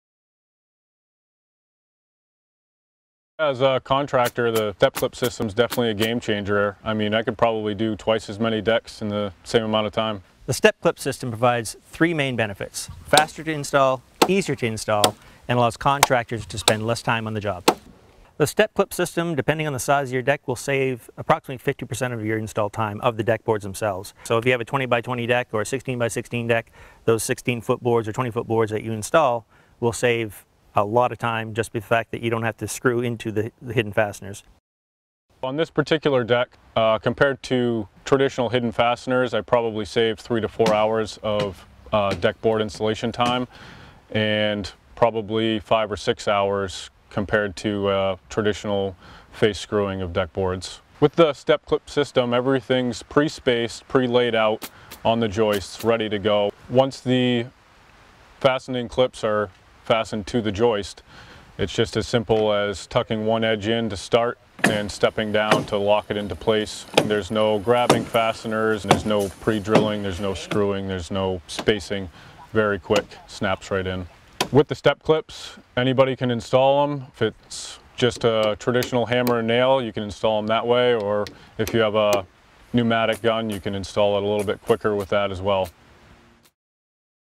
Veranda Featuring Step-Clip: Deck Installation System
Build your dream deck in HALF the time! Up to 50% faster than other hidden fastener installation methods.